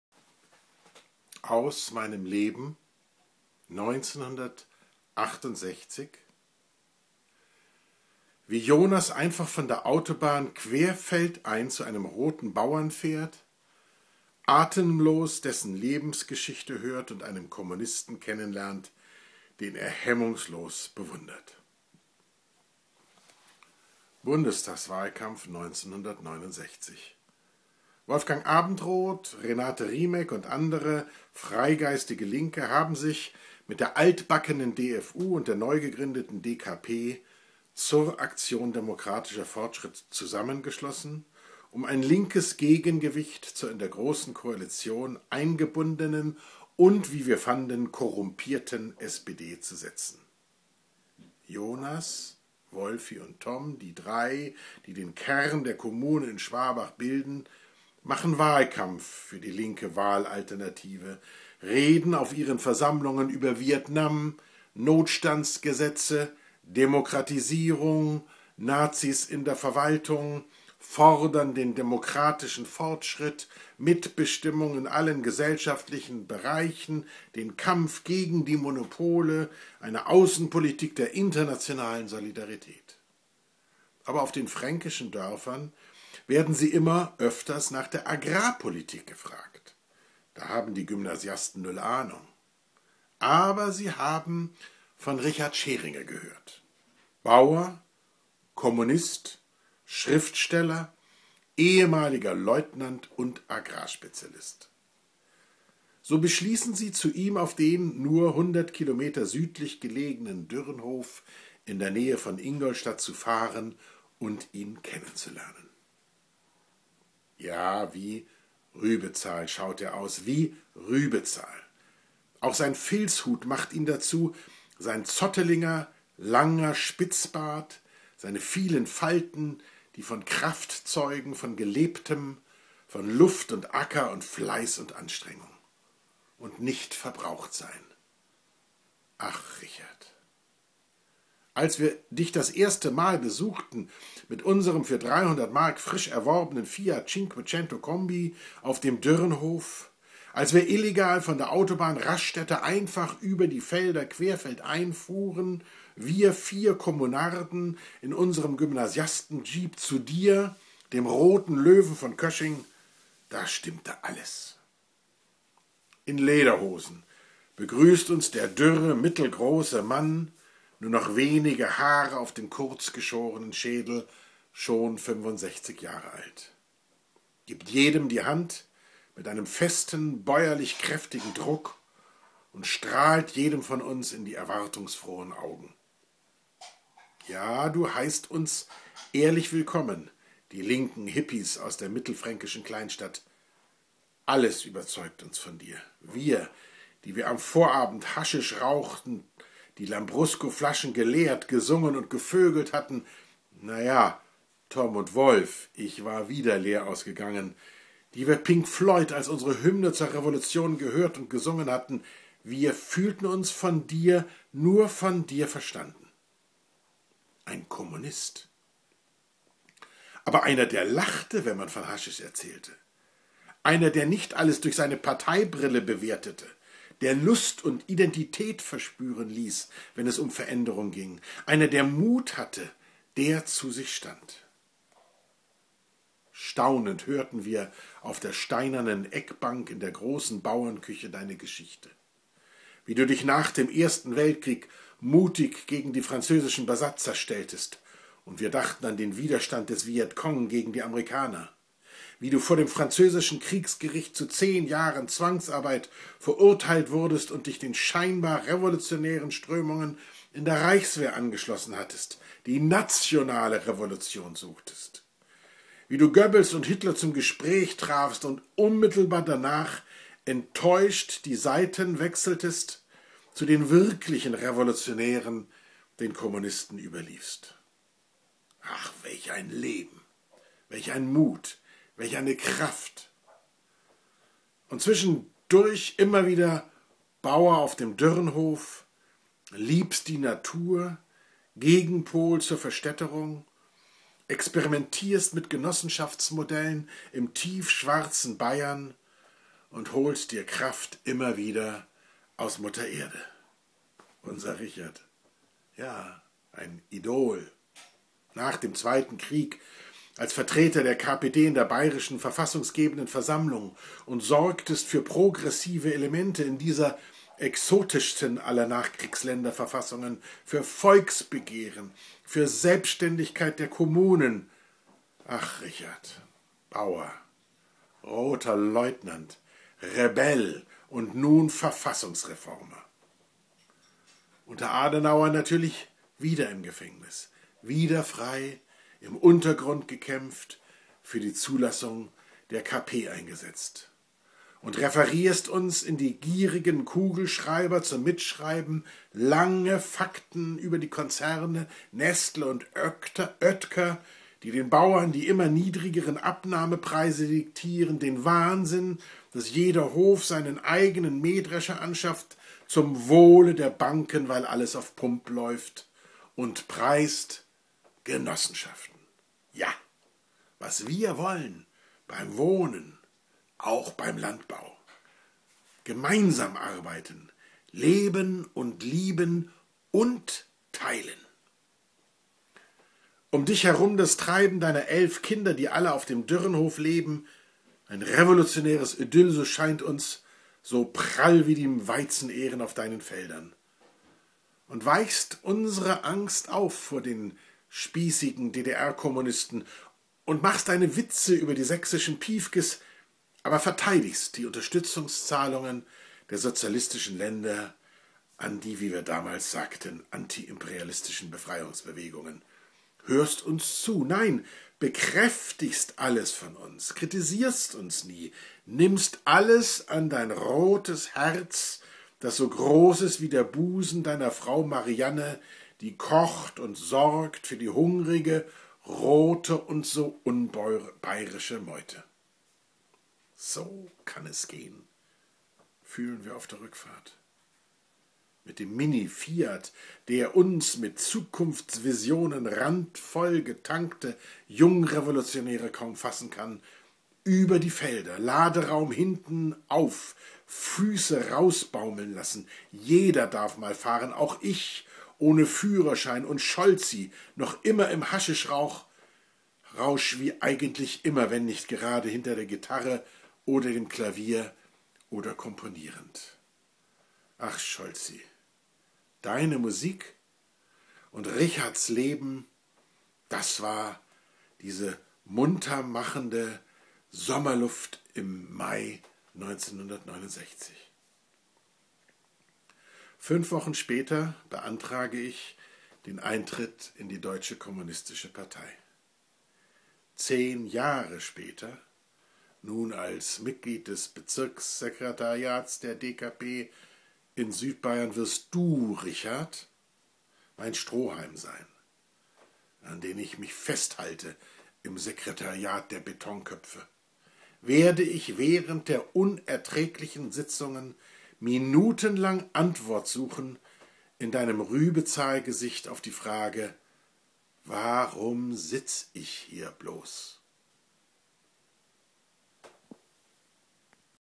Bei RICHARD SCHERINGER, den "Roten Löwen von Kösching", zu Besuch. 1969.
"Wie Jonas einfach von der Autobahn querfeld ein zu einem roten Bauern fährt, atemlos seine Lebensgeschichte hört, und einen Kommunisten kennenlernt, den er hemmungslos bewundert. "